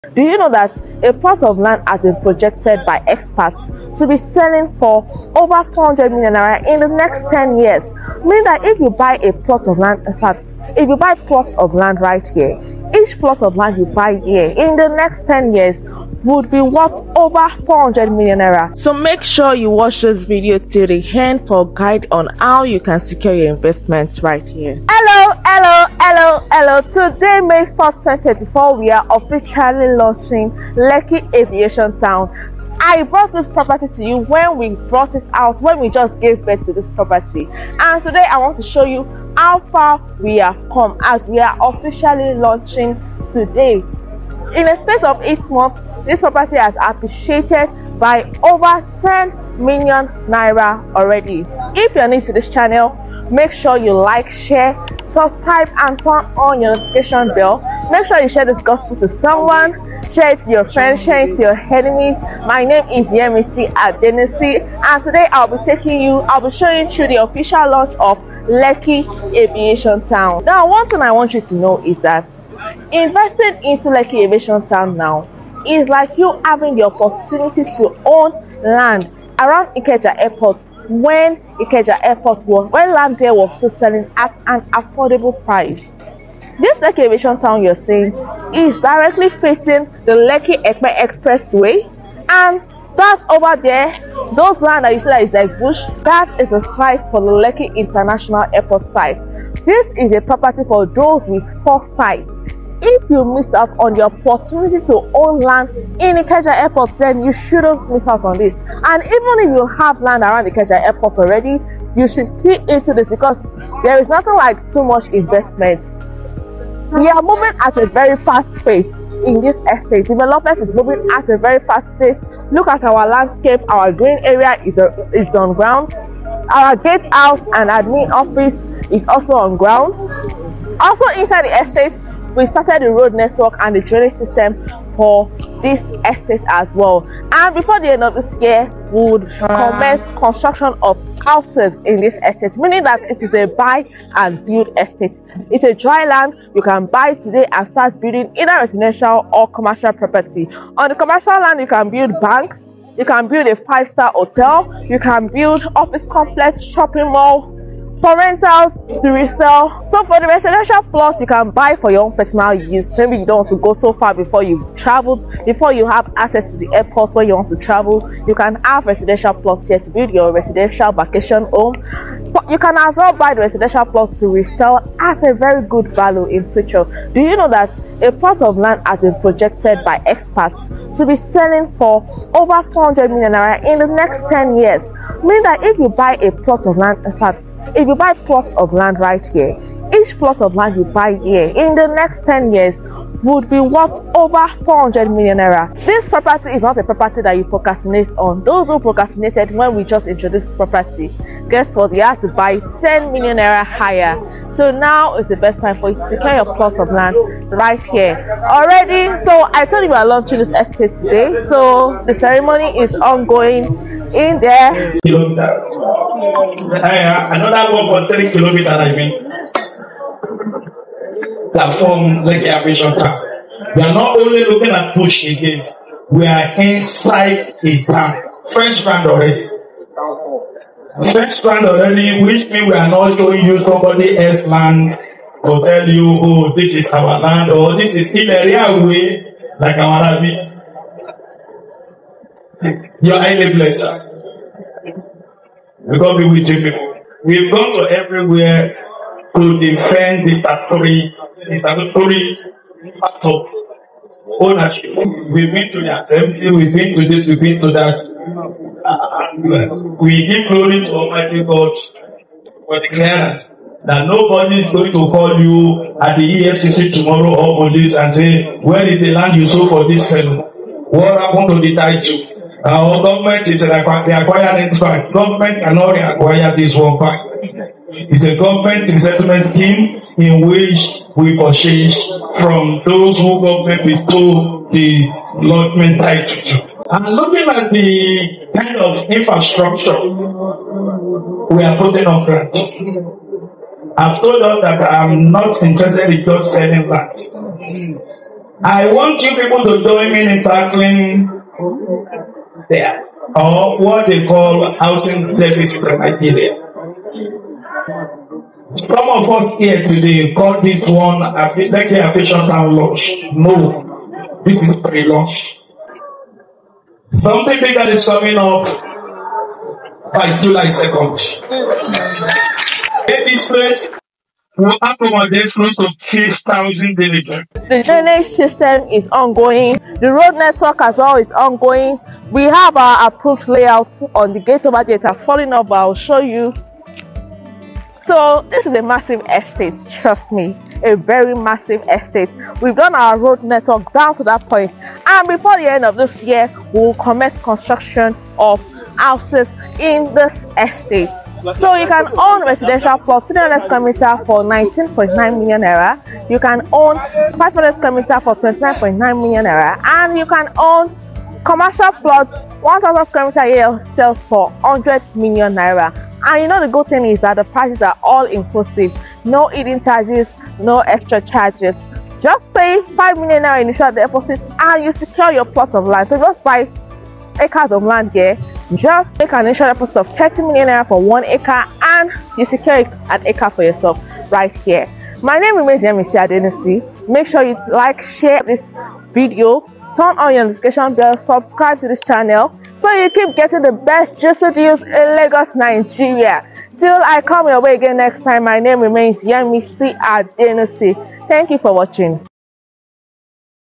Massive Investors Turnout for Land Sale in Ibeju-lekki at Lekki aviation town estate launch
Massive Investors Turnout for Land Sale in Ibeju-lekki at Lekki Aviation Town Launch

We still have land for sale in this estate situated in Ibeju-lekki, Lagos.

You can get commercial and residential plots here.

PRICE:
Commercial plots:
1000sqm for ₦100,000,000

Residential plots:
300sqm: ₦15,000,000
500sqm: ₦25,000,000

TITLE:
Certificate of Occupancy

FEATURES:
-Helipad
- Asphaltic road network
- Good drainage system
- 24/7 Electrification
- 24/7 Standard Security
- Recreational center
- Landscaping
- 100% dry land would be delivered etc.